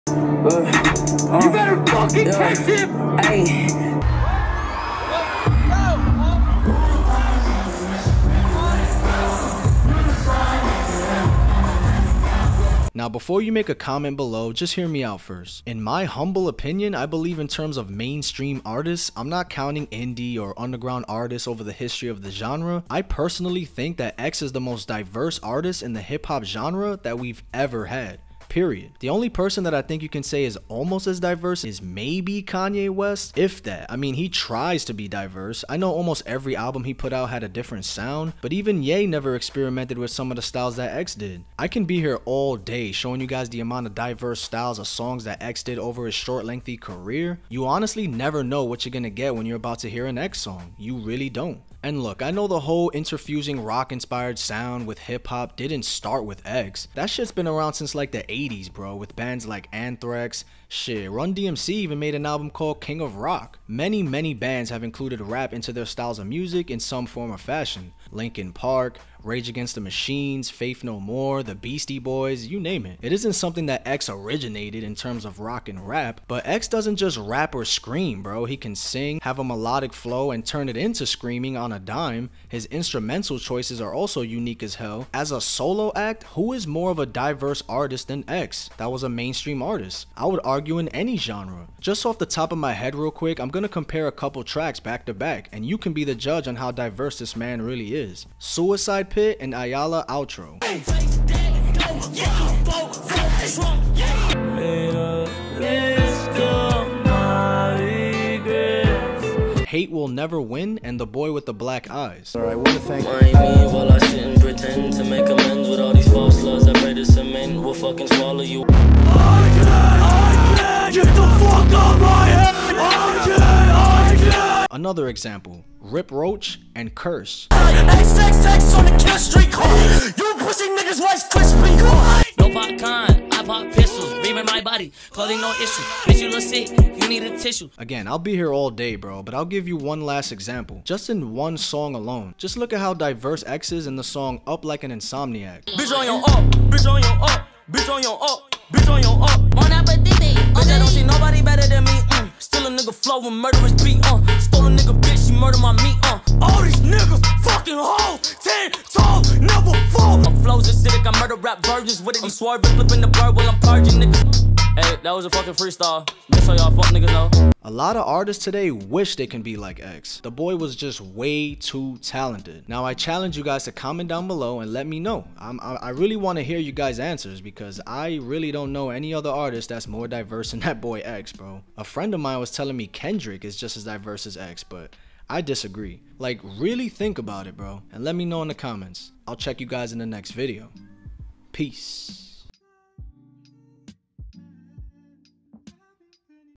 XXXTentacion is The Most Diverse Artist In Hip Hop History
Lets really talk about it fam.